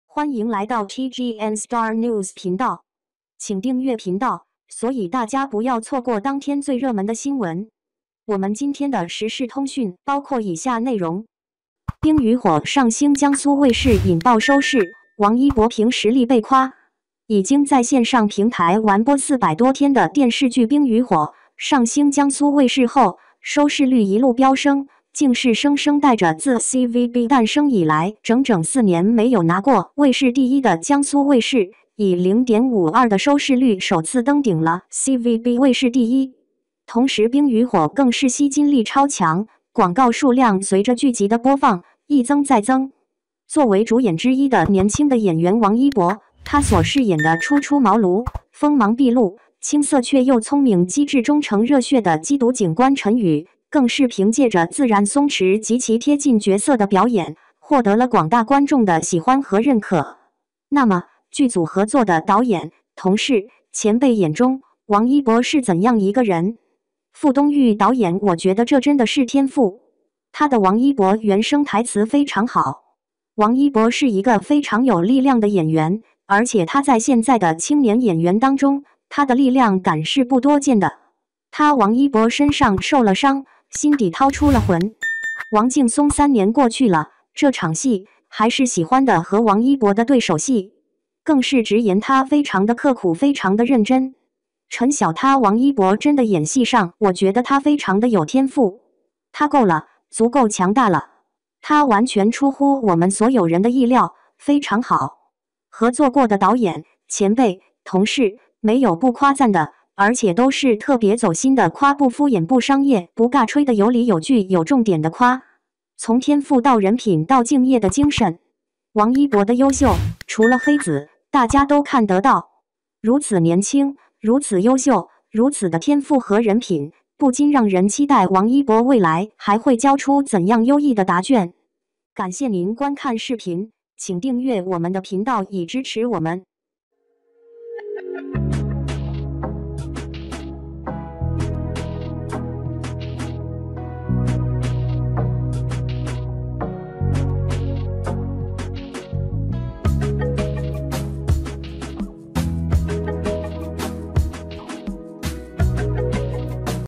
《冰雨火》上星江苏卫视引爆收视，王一博凭实力被夸
《冰雨火》上星江苏卫视引爆收视，王一博凭实力被夸 | TGN Star News